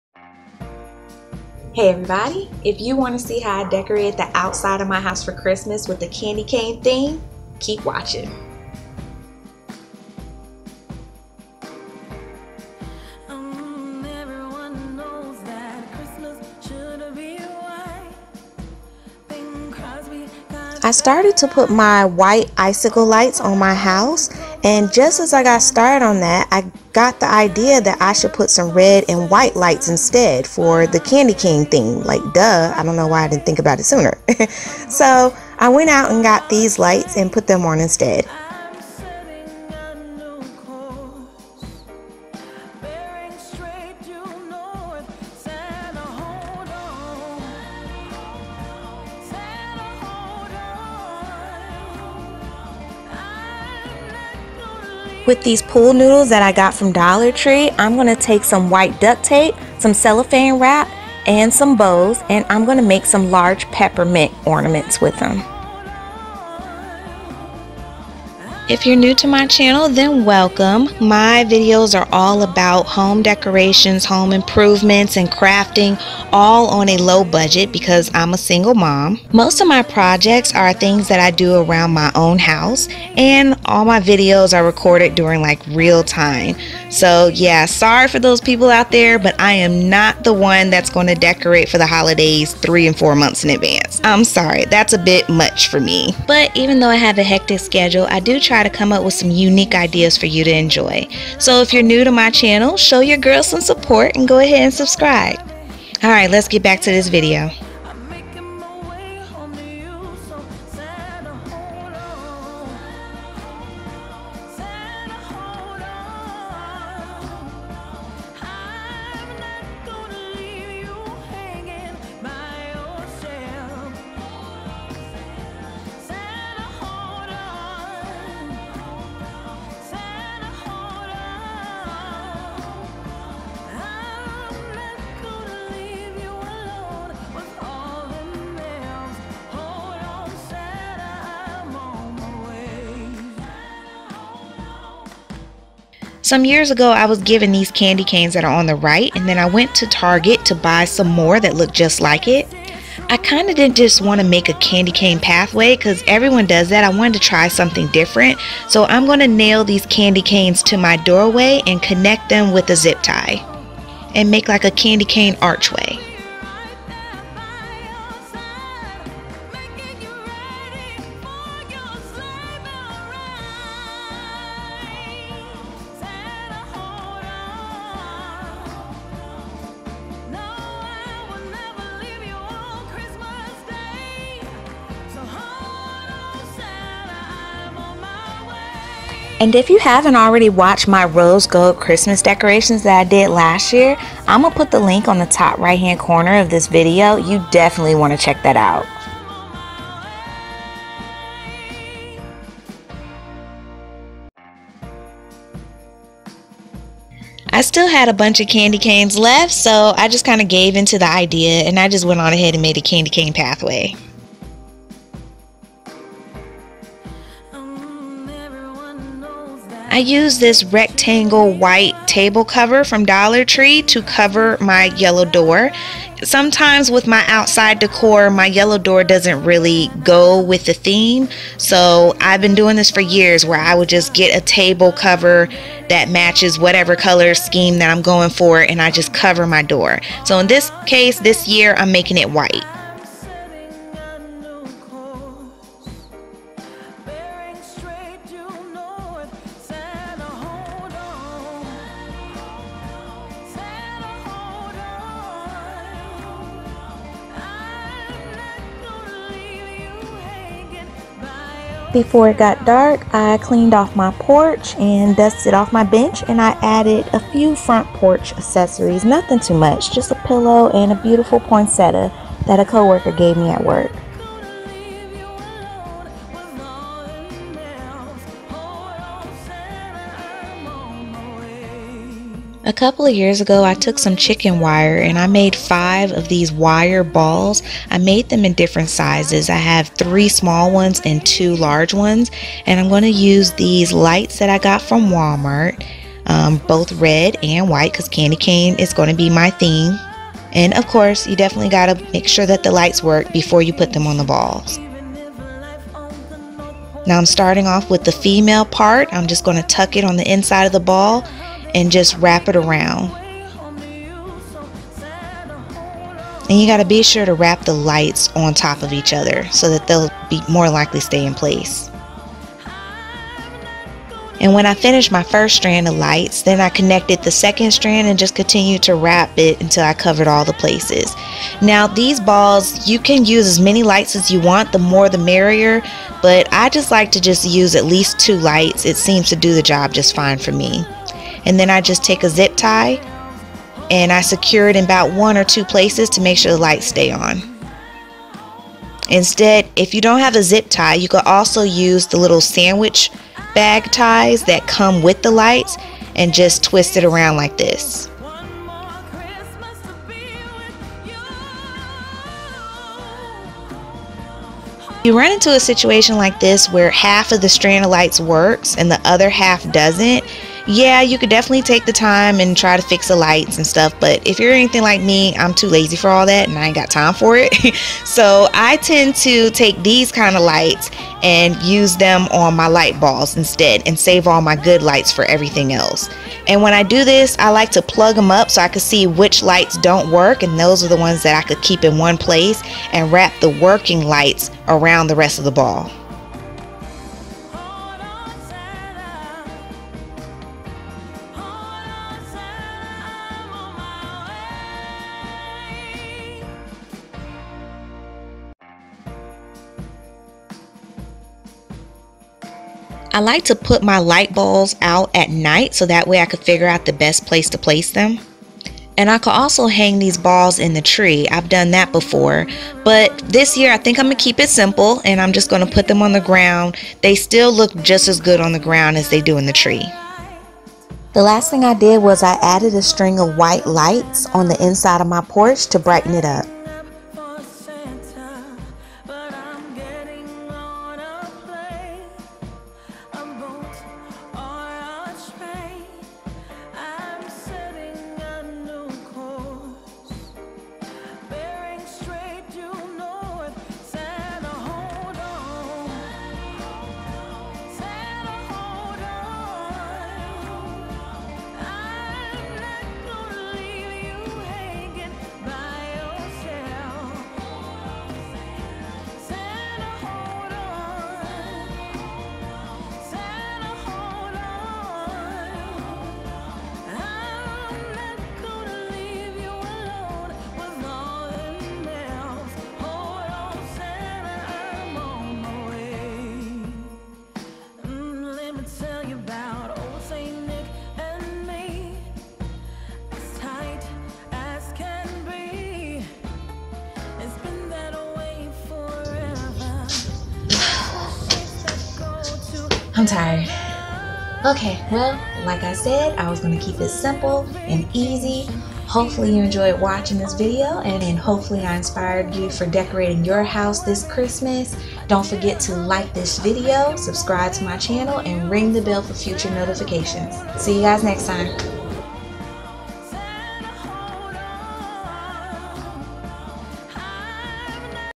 CANDY CANE OUTDOOR CHRISTMAS LIGHTS & DECORATIONS ~ DECORATE WITH ME
This year I decided to decorate outside of my house with a candy cane theme. I kept it easy and simple.